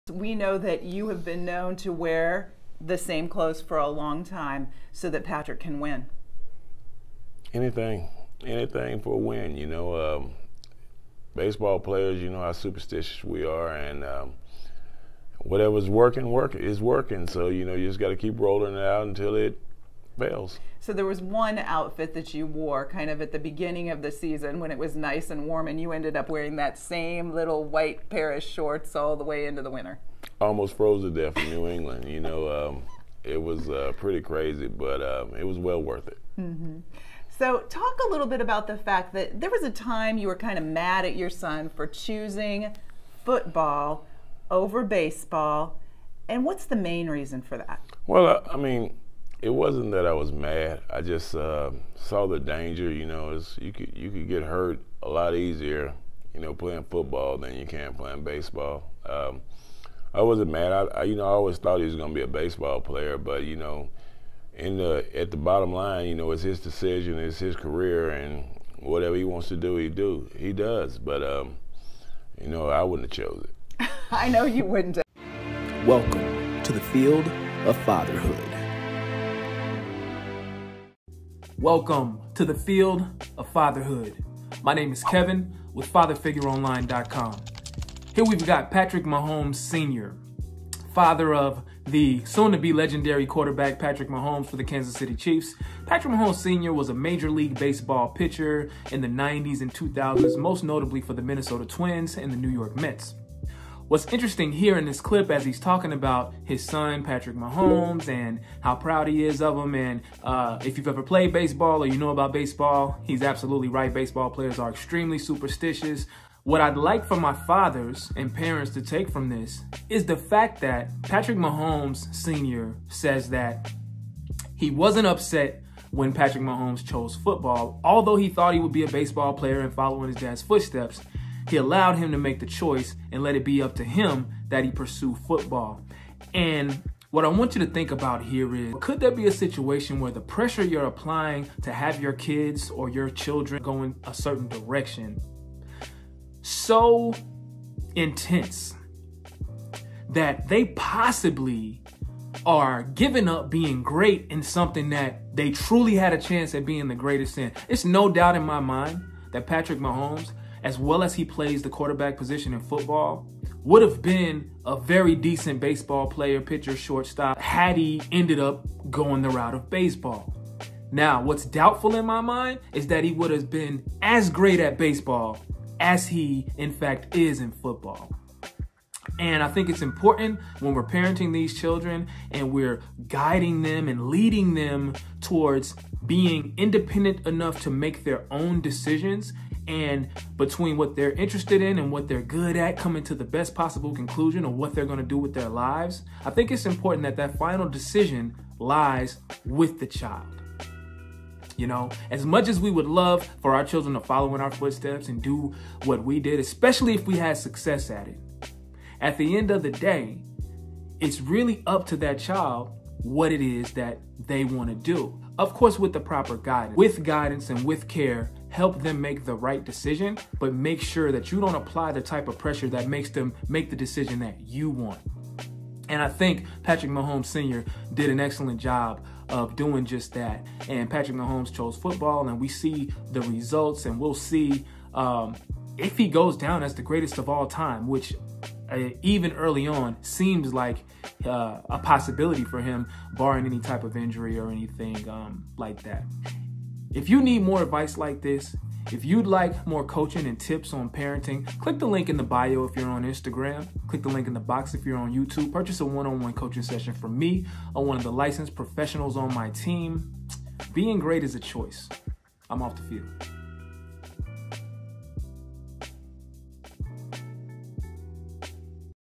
Patrick Mahomes Dad on Him Choosing Football
In this clip, Patrick Mahomes Sr. expresses how he feels about his son, legendary QB Patrick Mahomes II choosing football over baseball which he played professionally himself.

Often as parents, we lead our children into decisions based more on our interests than their own which can lead to regret later in life especially if windows close on certain opportunities based on the parent’s steering.

Have you ever made a decision opposite of your true feelings based on a parent or loved one’s fear/advice??

CLICK LIKE

CLICK SAVE

CLICK SHARE

Do you want more fatherhood and parenting advice?